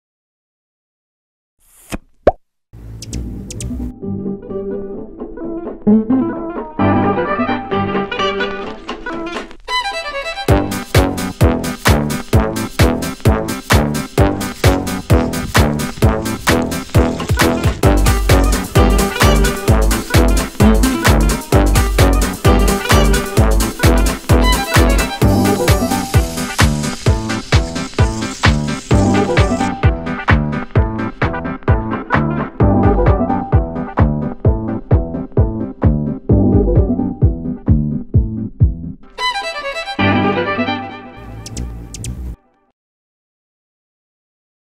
Odyssey Toys' Meddling Mouse
Remote controlled animals usually are the most fun to play with, since they resemble and mimic lifelike creatures. The RC mouse is no different – watch as this super fun critter rapidly spins around, keeping whoever watches it mesmerized and entertained!